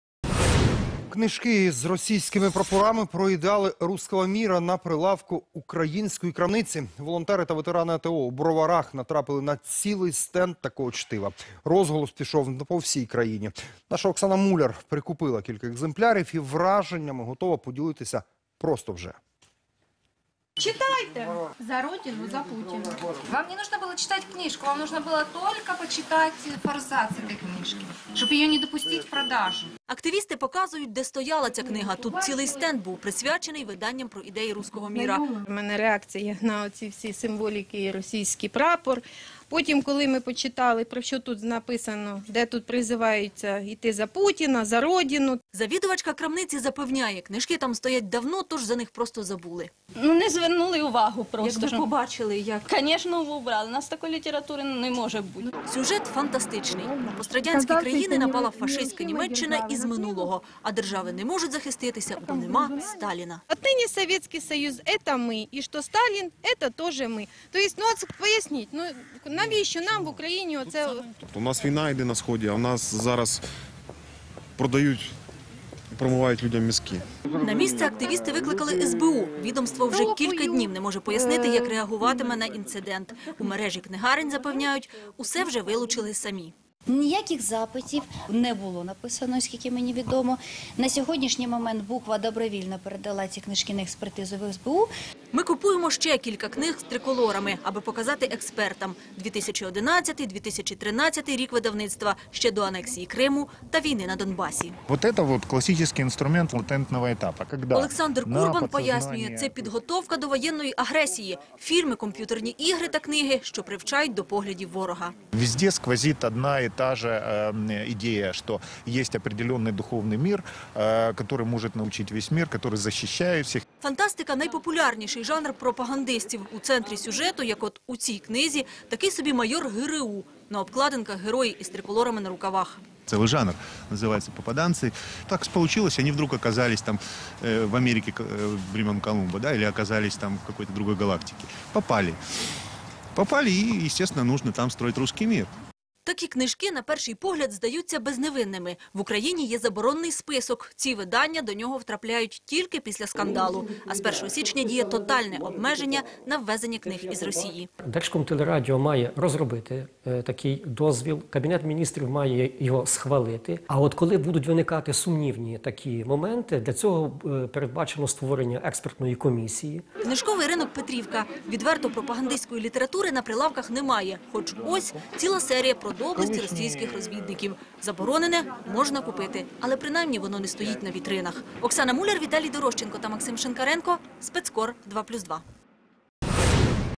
Книжки із російськими прапорами про ідеали "руского міра" на прилавку української крамниці
Волонтери та ветерани АТО в Броварах натрапили на цілий стенд такого чтива. Розголос пішов по всій Україні.